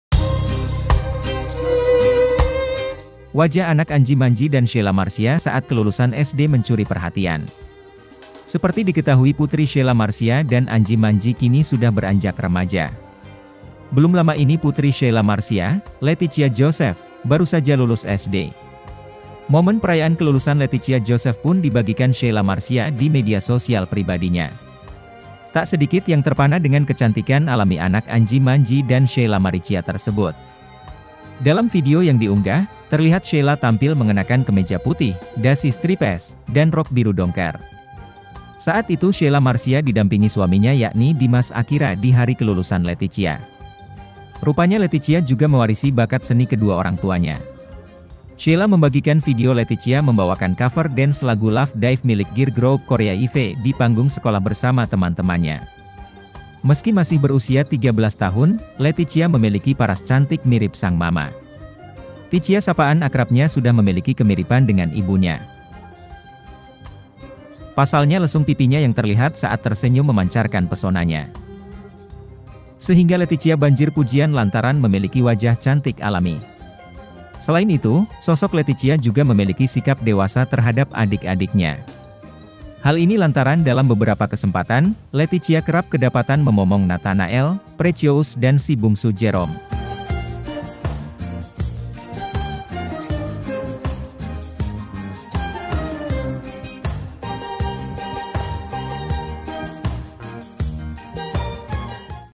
Wajah Anak Anji Manji dan Sheila Marcia Saat Kelulusan SD Curi Perhatian
Wajah anak Anji Manji dan Sheila Marcia saat kelulusan SD mencuri perhatian.

Seperti diketahui putri Sheila Marcia dan Anji Manji kini sudah beranjak remaja.

Belum lama ini putri Sheila Marcia, Leticia Joseph, baru saja lulus SD.